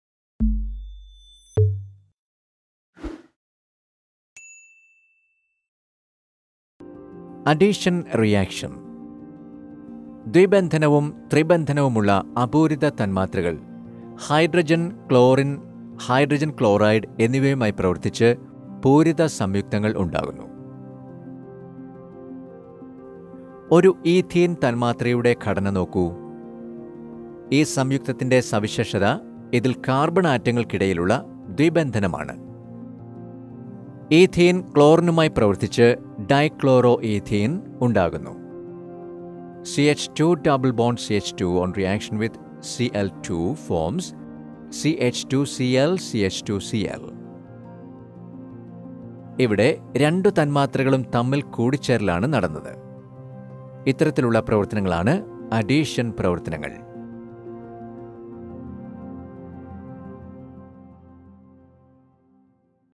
Addition Reaction:Organic compounds and reactions:Chemistry
In Addition reaction molecules with double and triple bonds can combine with molecules such as H2,Cl2,HCl etc. As a result of this, compounds with double bond get converted in to saturated compounds and compounds with triple bond get converted first into unsaturated compounds with double bond and then to saturated compounds with single bonds.
See the reaction between ethene and chlorine.
The special feature of this compound is double bond between the two carbon atoms. Ethene reacts with chlorine to form 1,1- dichloro ethane.
CH2 = CH2+Cl                CH2Cl- CH2- Cl